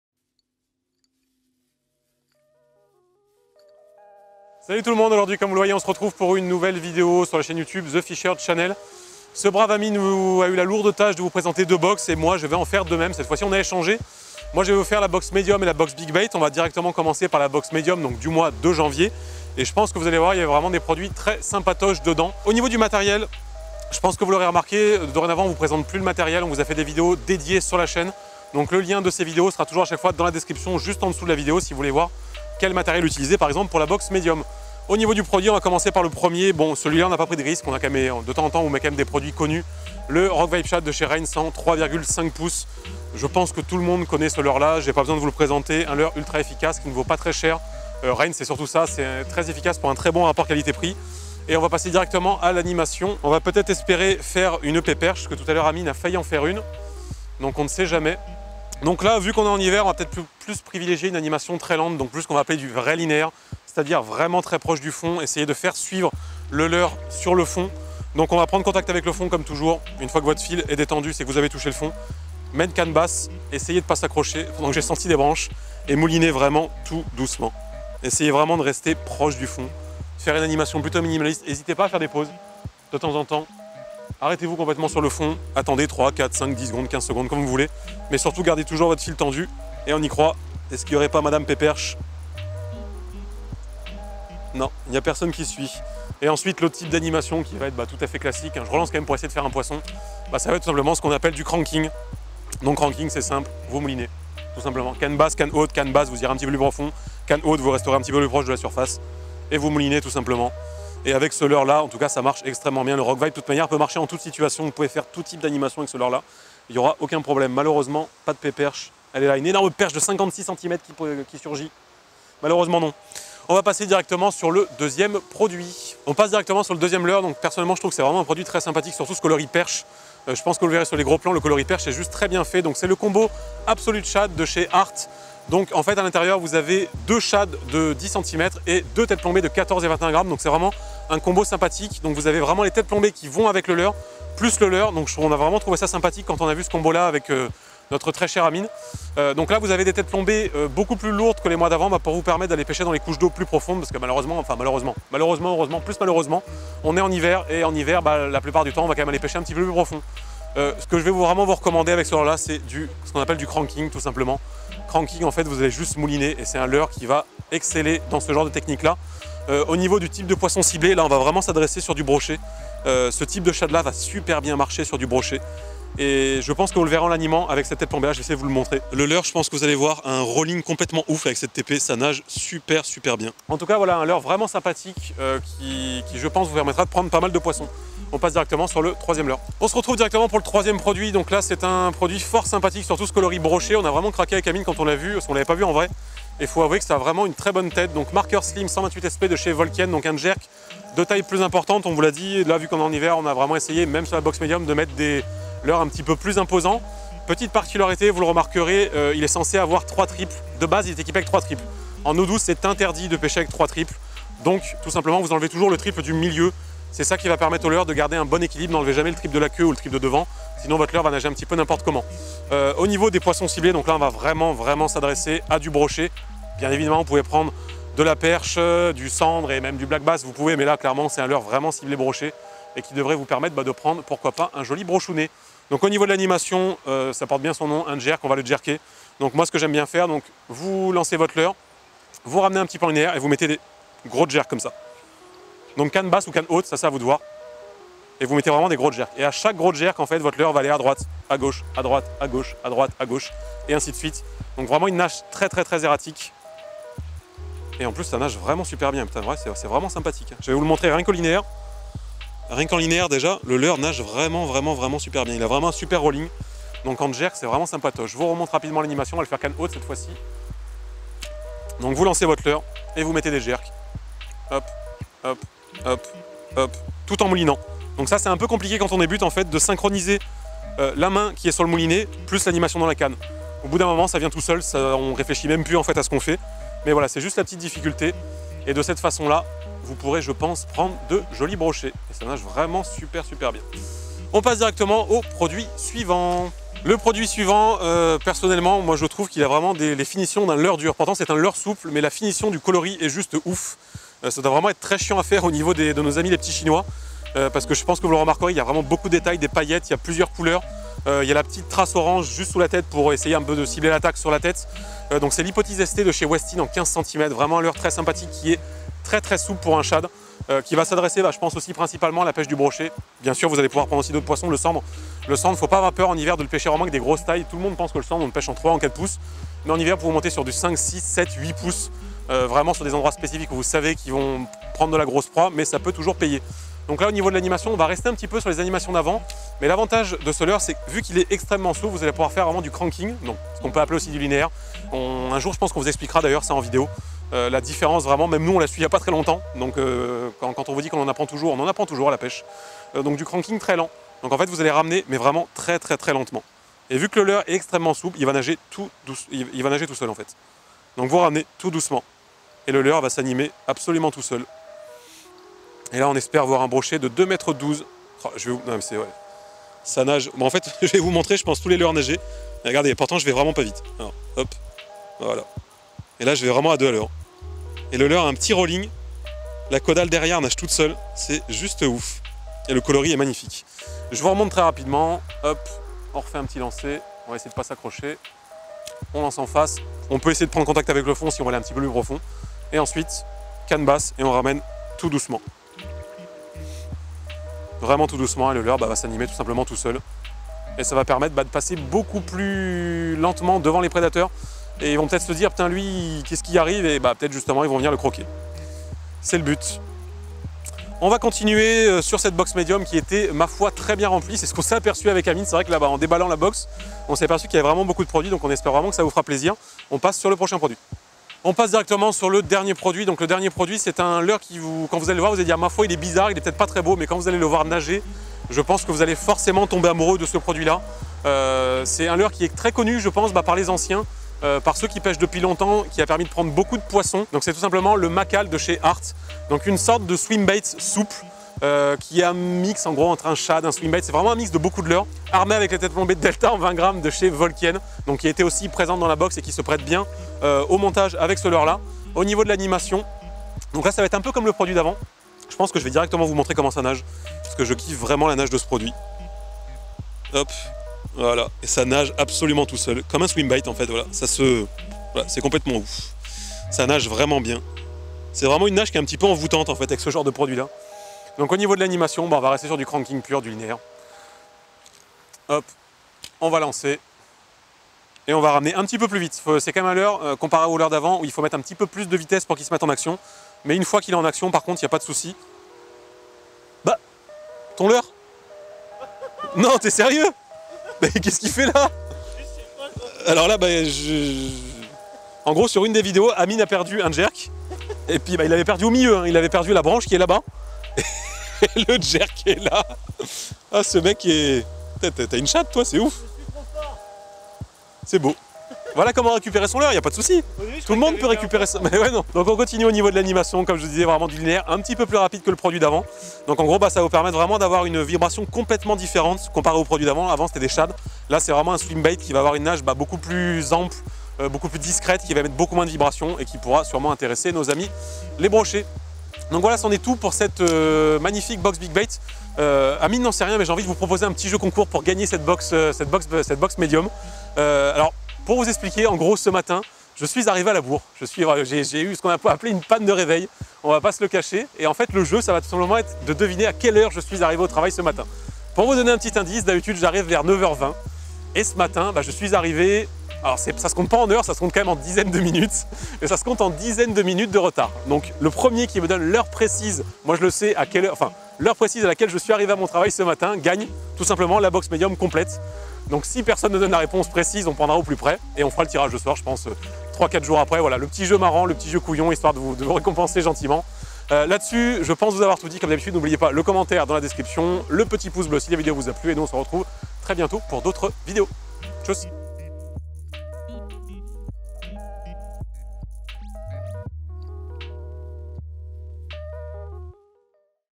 Tuto pour ANIMER vos LEURRES de la FISHER BOX Médium du mois de janvier
Kevin vous montre comment animer et pêcher avec les produits présents dans la Fisher Box Médium du mois de janvier.

Vidéo du matériel pour les Box